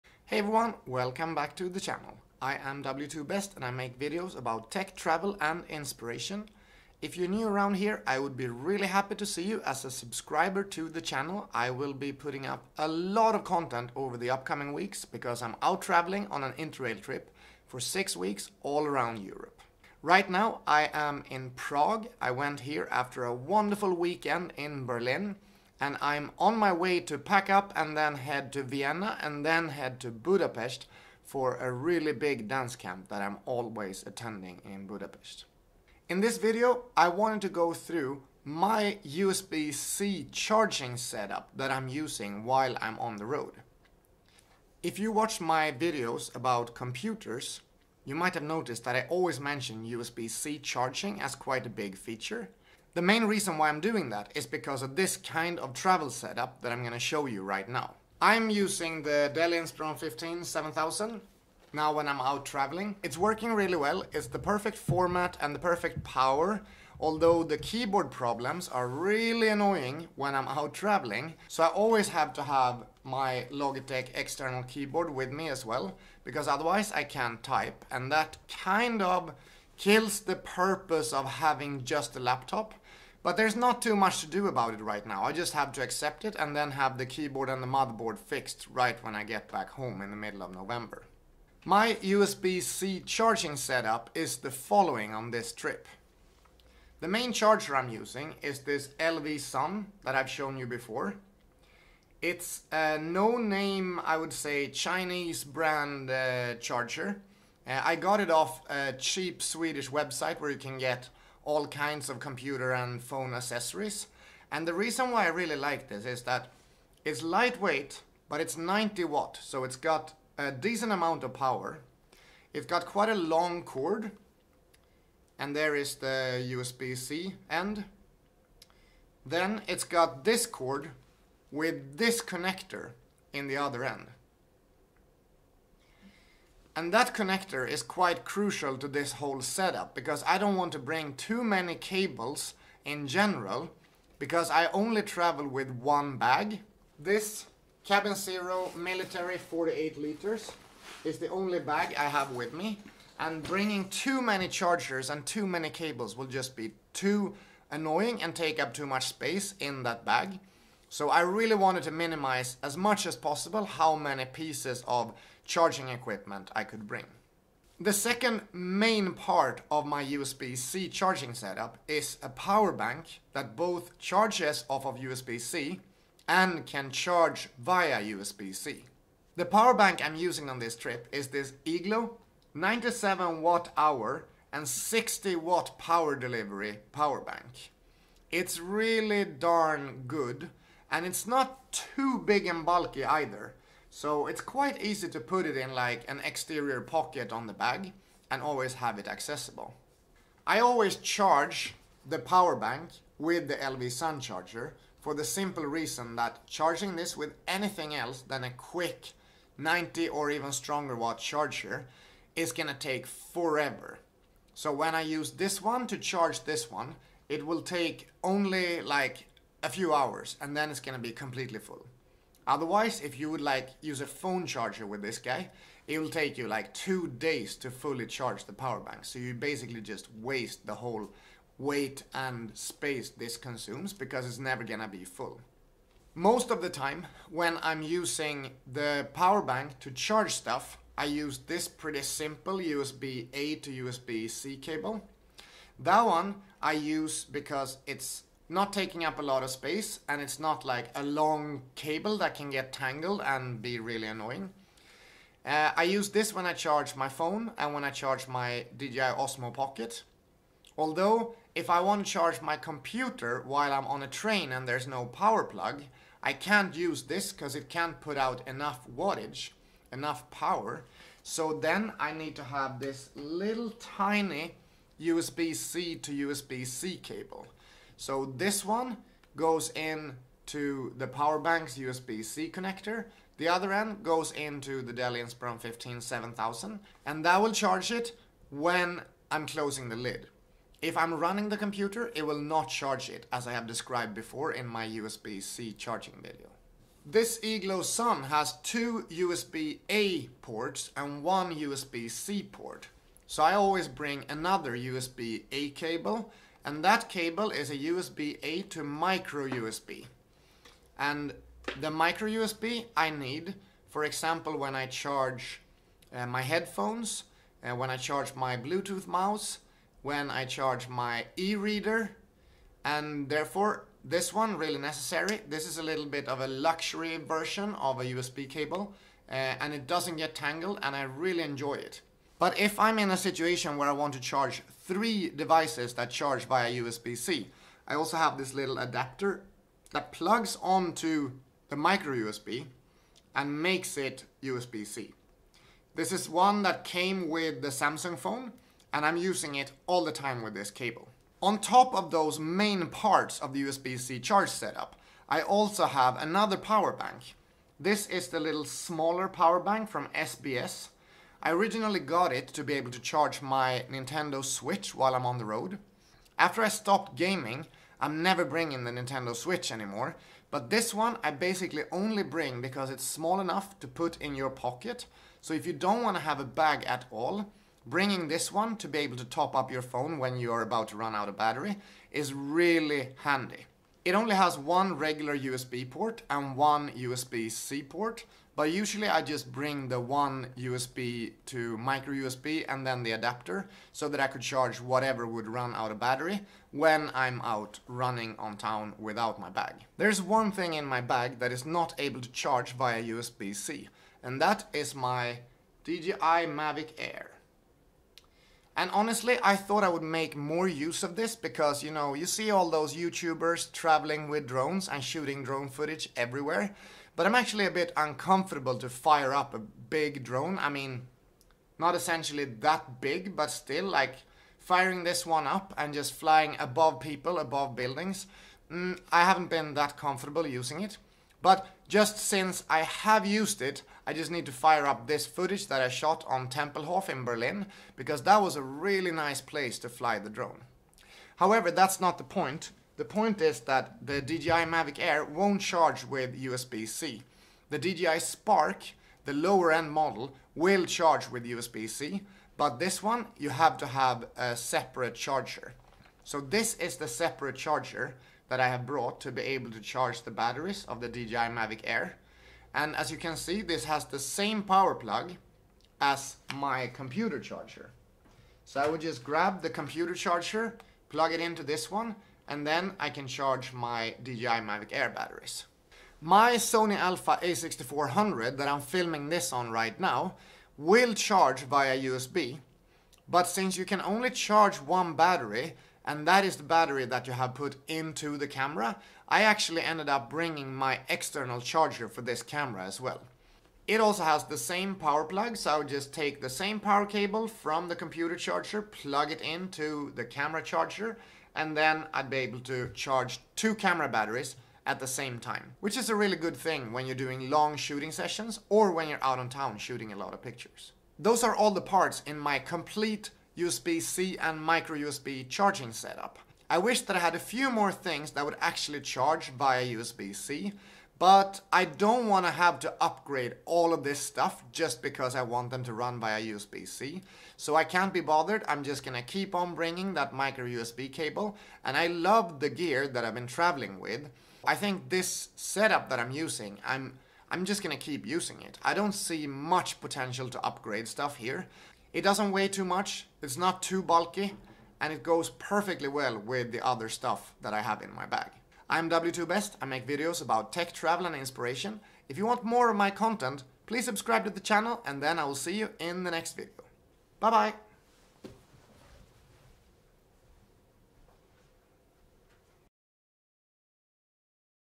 USB-C Charging Setup for Travel (Power Brick, Power Bank & Cables)
I always mention USB C charging as important when it comes to laptops and this is the reason why. I really want to be able to have one charger and one power bank that can take care of all my power needs on the road. In this video I go over my complete charger setup for being on the road.

My Gear:
2 TB Extra SSD
Main Camera
Variable ND-Filter
Bring Everywhere Camera
18mm Lens Addon
Mic / Audio Interface
On Camera Light
Laptop Stand